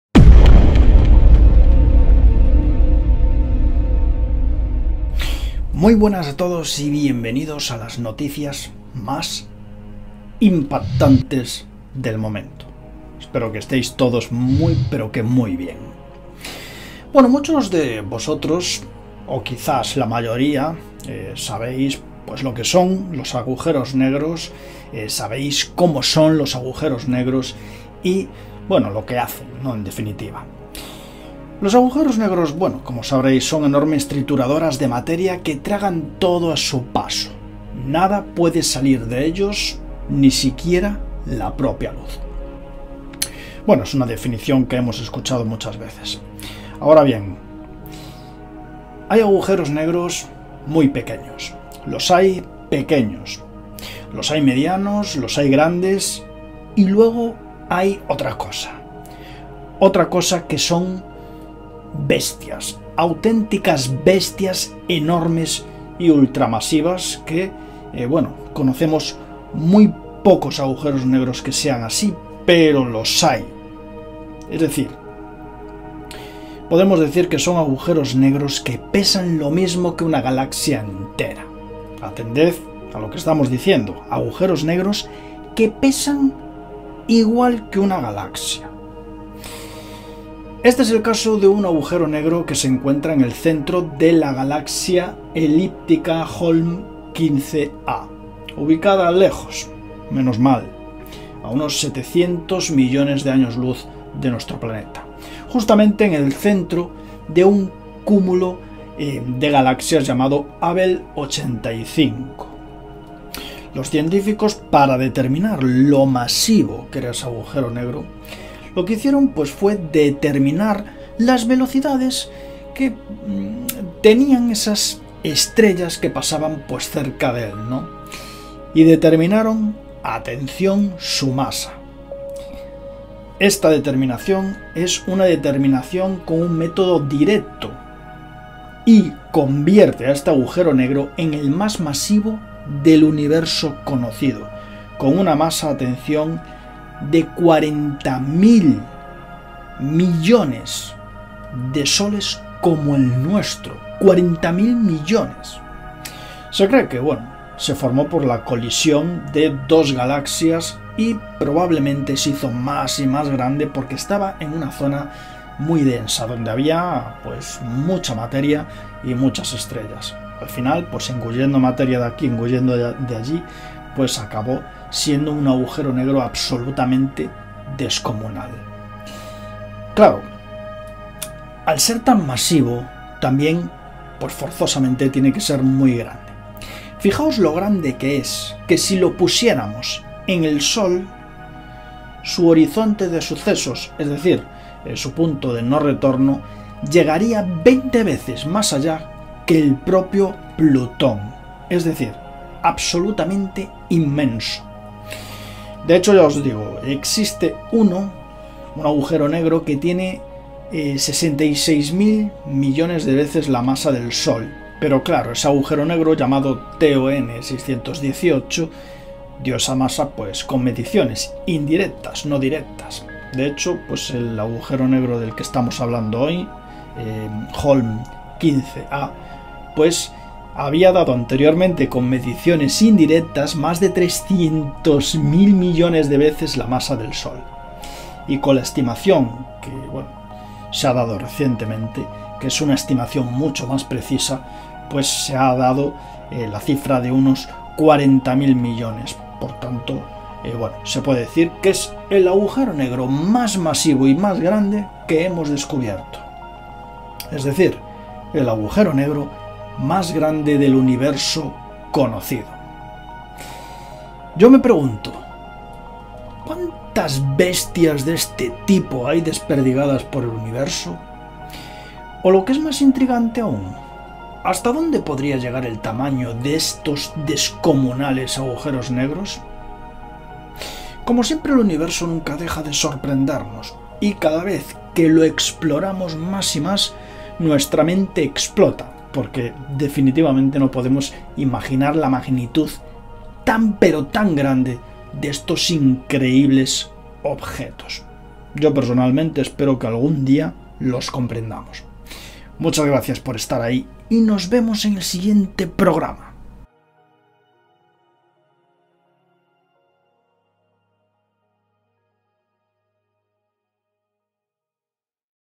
Descubren el AGUJERO NEGRO Más GRANDE del Universo Conocido
En agosto de 2019 se daba a conocer el agujero negro más grande del universo conocido, comprobado por estimaciones directas. El agujero negro es tan masivo como 40 mil millones de soles como el nuestro y si lo colocáramos en nuestro sistema solar llegaría 20 veces más allá de Plutón…

Atribuciones: NASA / CXC / SAO / A.Vikhlinin et al. /SDSS